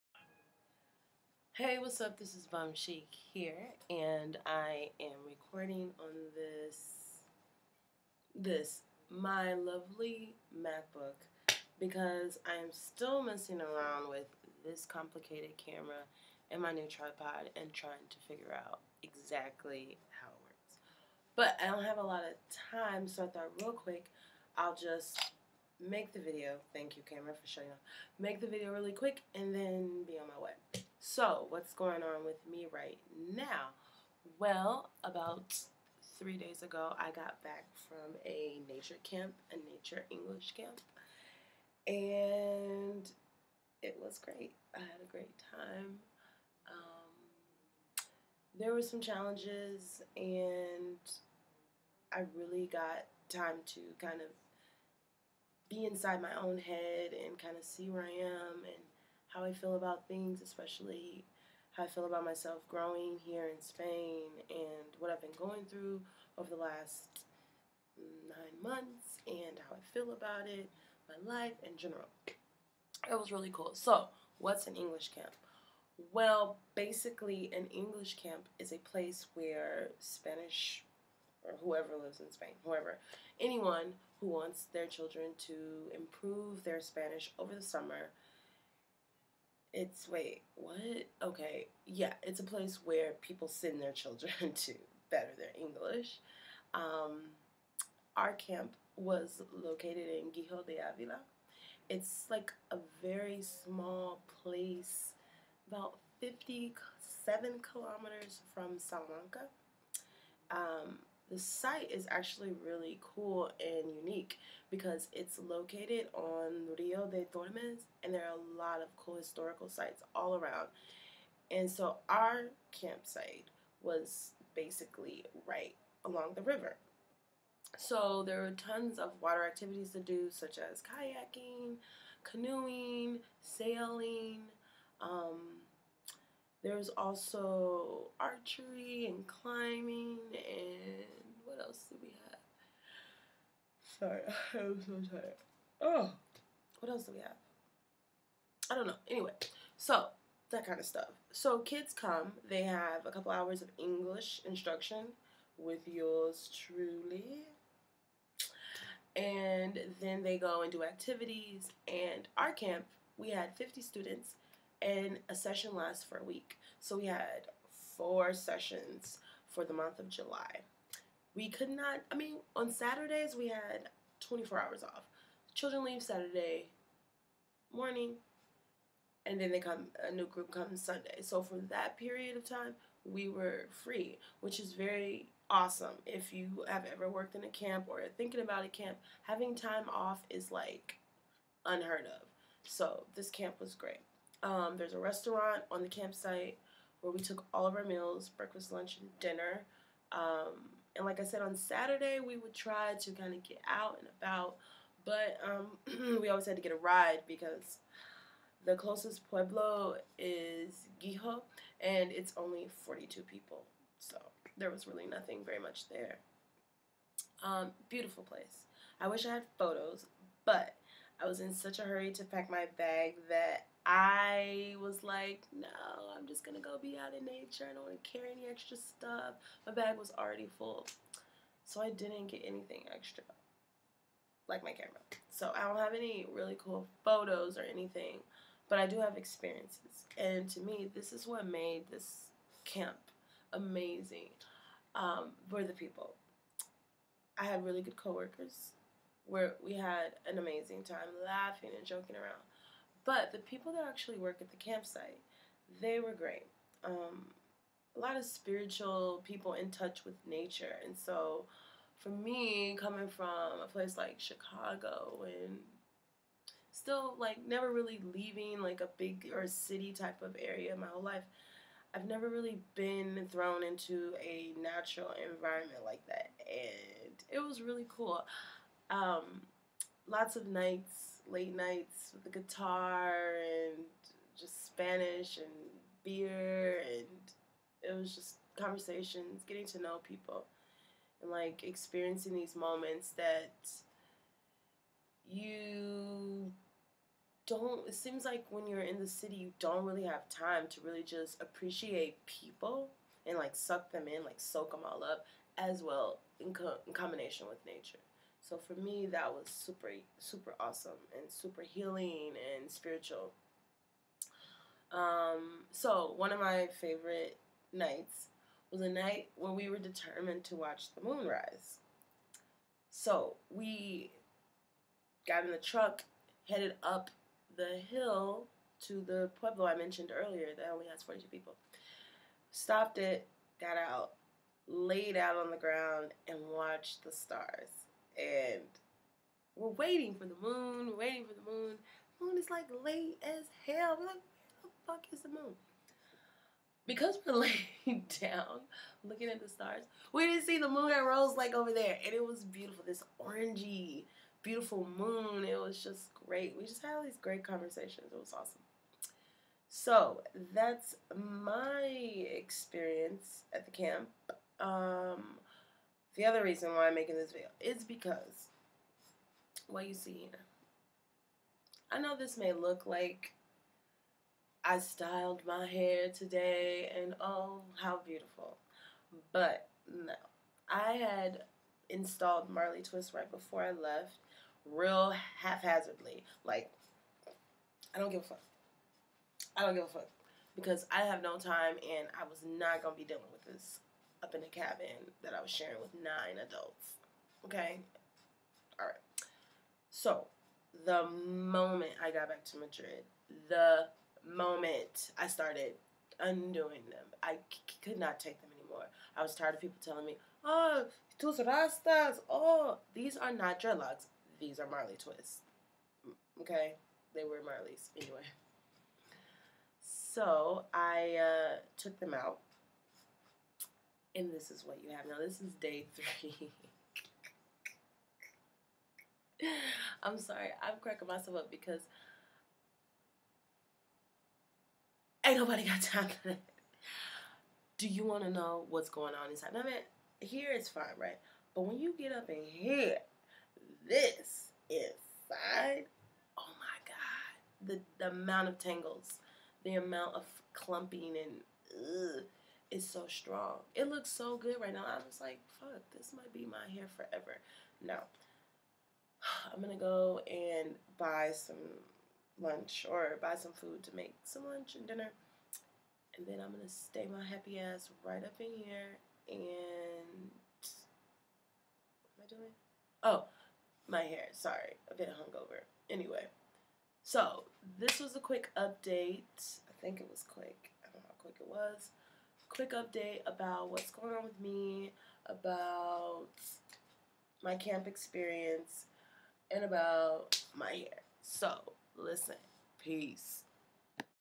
English Camp in Spain
I worked at an English Nature Camp for the month of July